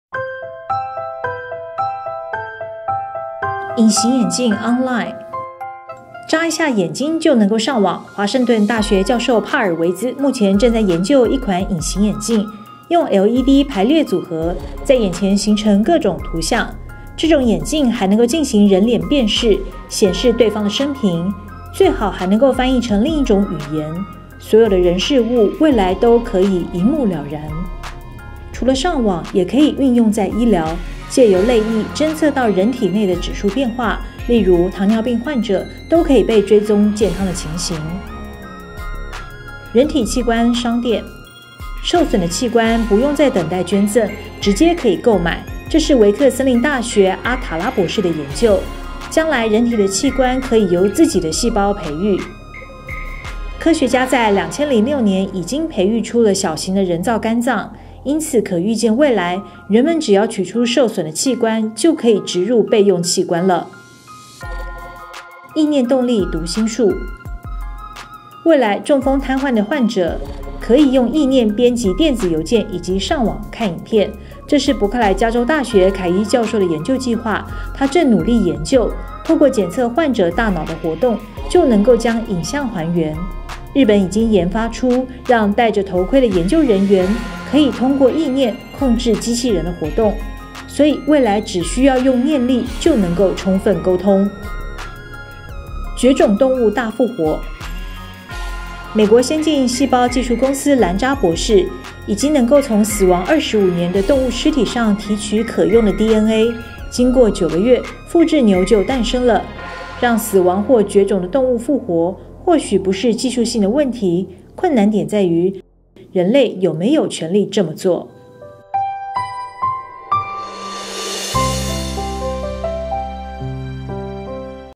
【味全TV】預見2030
到了2030年，我們的隱形眼鏡可以上網，人體的器官可以自行購買，意念動力可以幫助癱患者和大眾溝通，這些都不是電影裡的情節，而是真的有學術單位在進行的研究計畫，依循這些脈絡，味全TV帶您預見健康大未來。更多請見：（